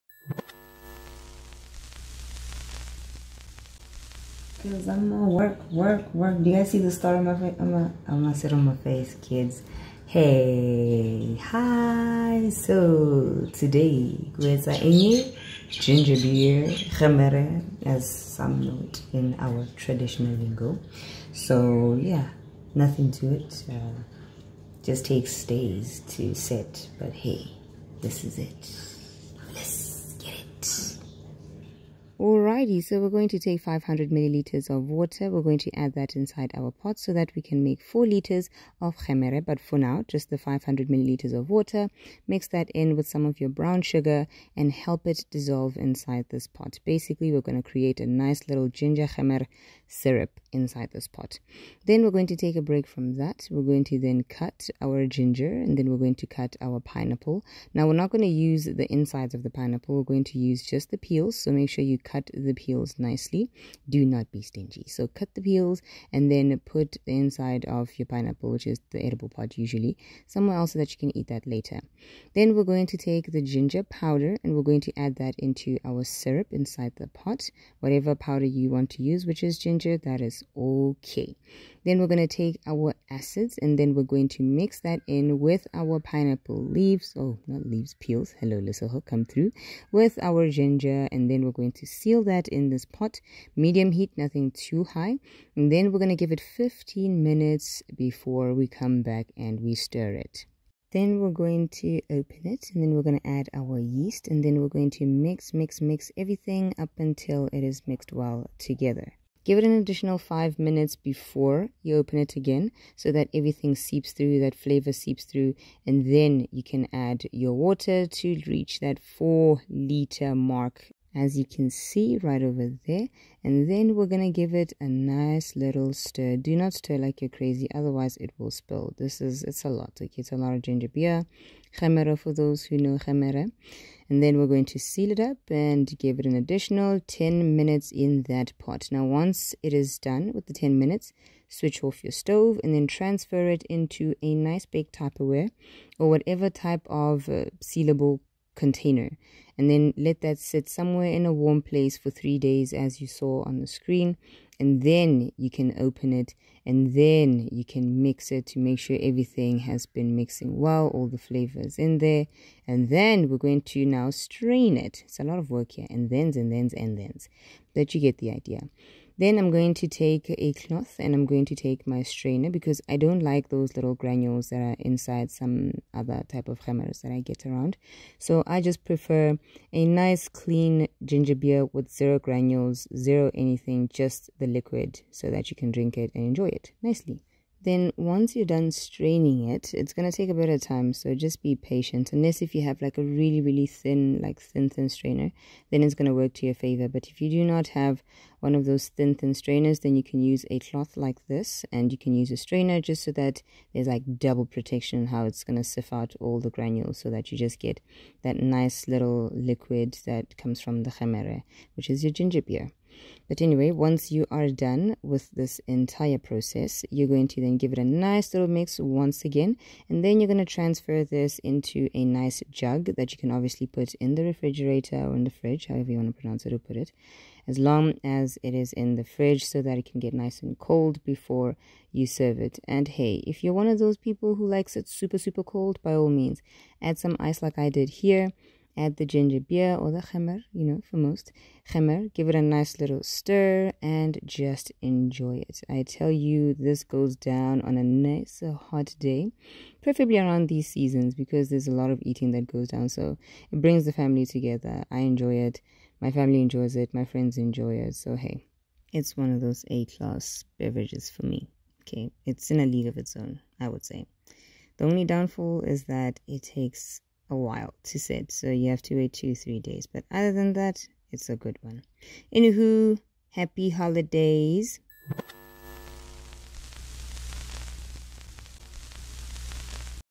Traditional Ginger Beer *GEMERE* || SOUTH AFRICA
Aloha amigos,

Follow on Instagram: @EatingwithLesego

INGREDIENTS:

- 3 1/4 Cups of Brown Sugar
- 4 Litres of Water
- X3 75g (150g) of Ground Ginger Powder
- 1 Raw Ginger
- 1/2 a Peel of Pinapple
- 1/2 a Cup of Raisins (Optional)
- 1 Tspn of Cream of Tartar
- 1 Tspn of Tartaric Acid
- 2 Tspns of Instant Dry Yeast

I sure hope that you will enjoy this simple but delicious piece of heaven.

Stay blessed.

Xoxo.